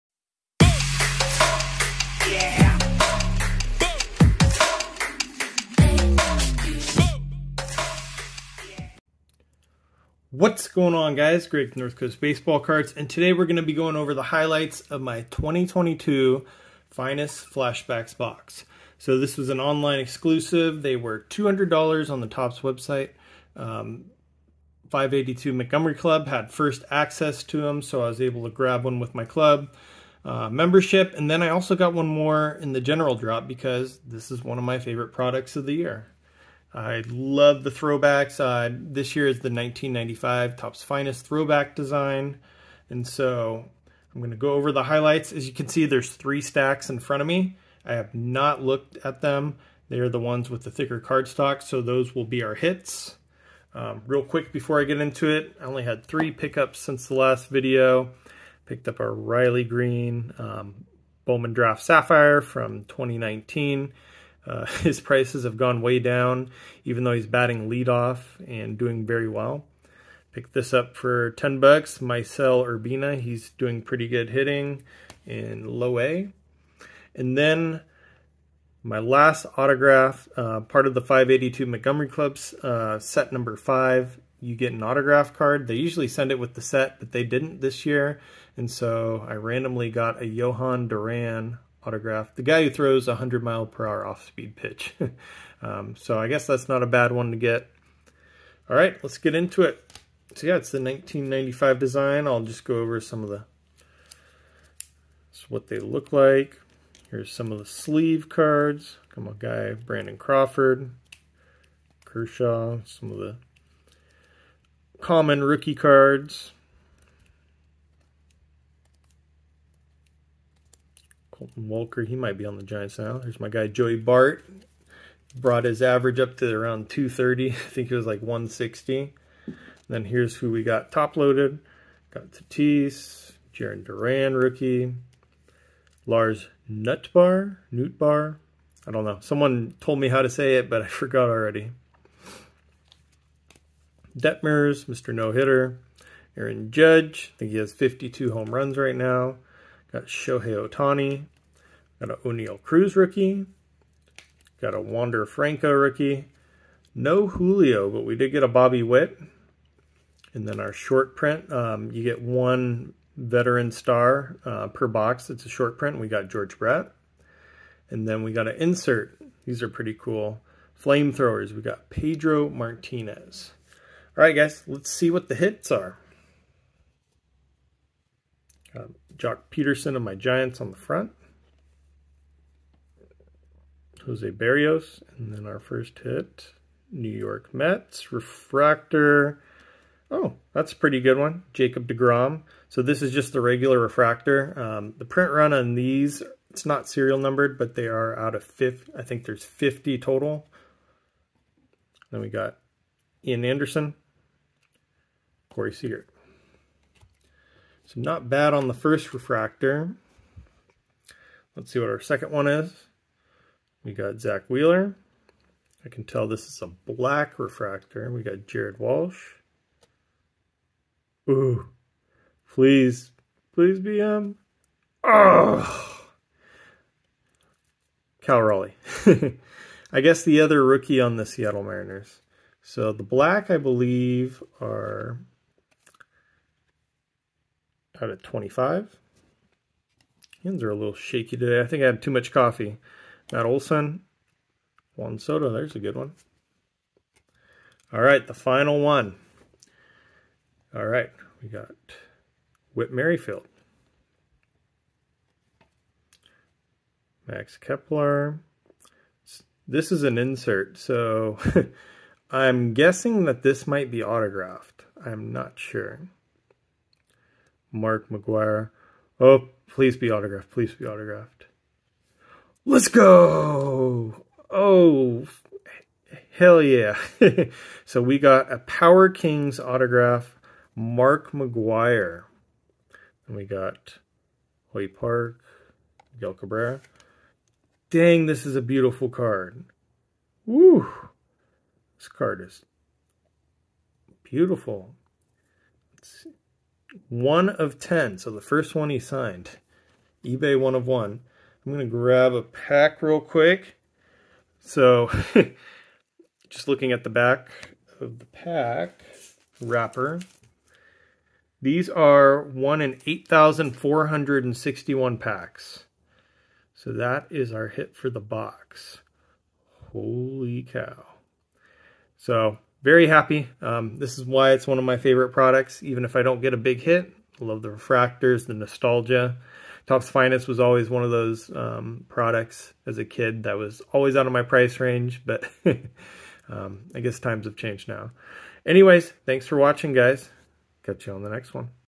New Product: 2022 Topps Finest Flashbacks Highlights (1 in 8,461) Auto Hit!!!!!! 😁😁
Today we go over the hits from my 2022 Topps Finest Flashbacks Box. Boxes containing a lot of packs I like to rip off camera and save the "hit" packs for the video so it is not a super long video.

Monster 1:8,461 auto hit 🔥